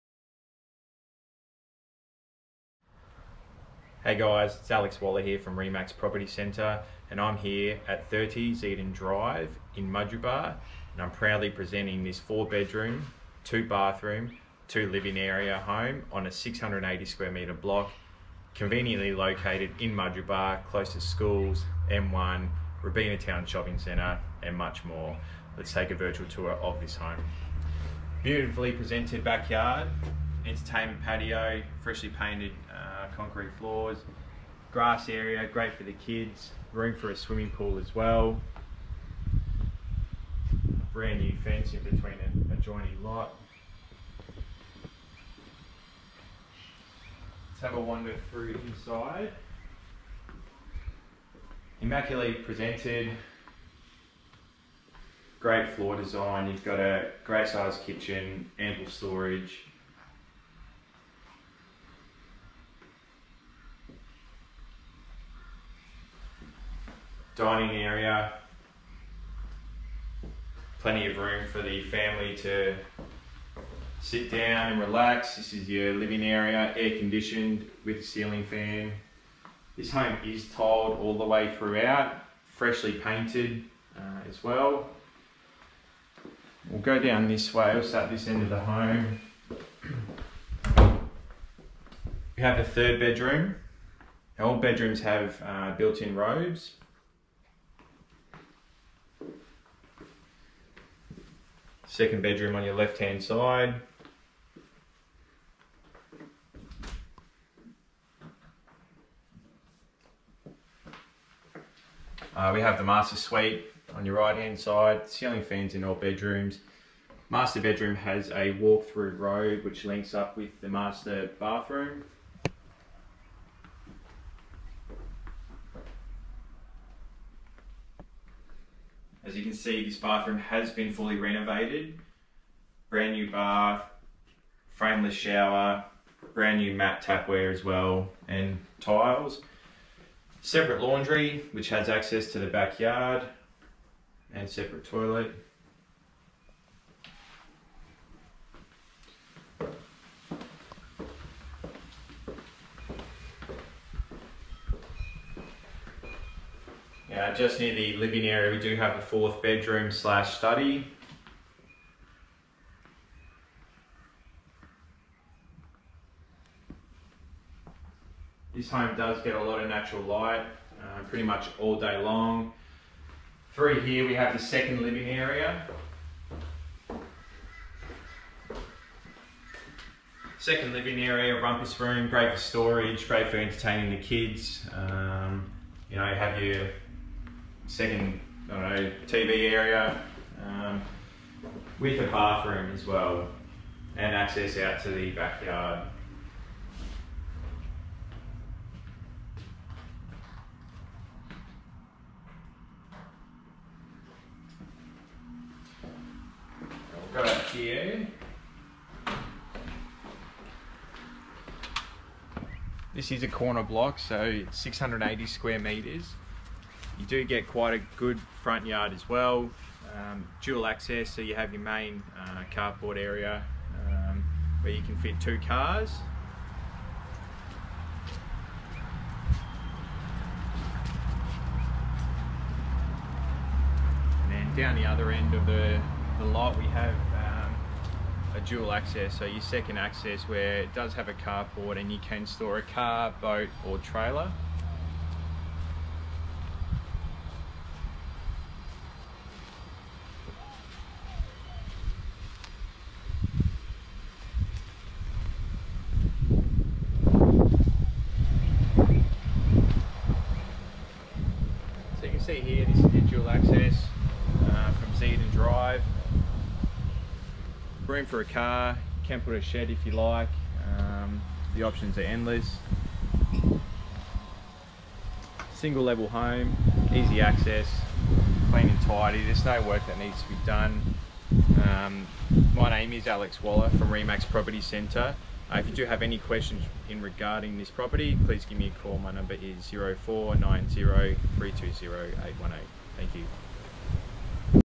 30 ZIEDAN DRIVE, MUDGEERABA
LARGE OUTDOOR ENTERTAINMENT AREA - CORNER BLOCK - CENTRAL LOCATION

Conveniently located within close proximity to Robina Town Centre, Somerset College, Mudgeeraba State School, Robina Hospital and easy access to the M1.

This low maintenance home is proportionally laid out offering generous indoor outdoor living spaces perfect for the growing family. Comprising of good sized living & dining area, well appointed kitchen with ample storage and seperate rumpus room providing extra space for the kids or home business.

Relax in your own private backyard, with an undercover patio making this home the perfect entertainer for friends and family.

Property Features:
* 3 Bedrooms plus study/4th bedroom.
* Master with walk through robe and access to bathroom.
* Full renovated two way bathroom.
* Large 2nd family/rumpus room with toilet, shower and external access. Can be shut off from the rest of the house and used for a home business. (*Please not this is not 2.4 metres high)
* Good sized kitchen with ample storage.
* Air-conditioned living area and ceiling fans in bedrooms.
* Seperate Laundry.
* Covered entertainment patio and plenty of yard for the growing family.
* Flat 680m2 block.
* Tiled throughout.
* Off Street Parking for 2 cars.
* Fully fenced block.
* Low maintenance gardens.
* Room to install a swimming pool.
* Brand new fence between adjoining lot.
* Corner block with dual access and room for a caravan, boat or trailer.

Location:
* Sporting fields and clubs - 2min Drive.
* Mudgeeraba State School - 2min Drive.
* M1 Access - 3min Drive.
* Somerset College - 6min Drive.
* Robina Town Centre, Station, Hospital, Stadium - 7min Drive.

For more information or to inspect this property please call Alex Wohler on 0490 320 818.